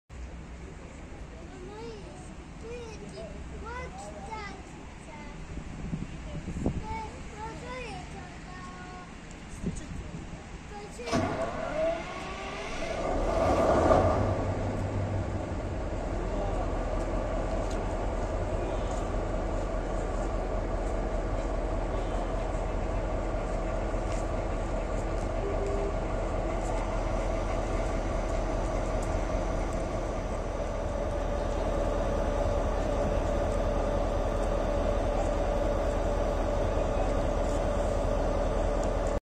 Hercules 1223 007 startup. Paljenje Herculesa 1223 007.
Zagreb Zapadni Kolodvor, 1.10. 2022.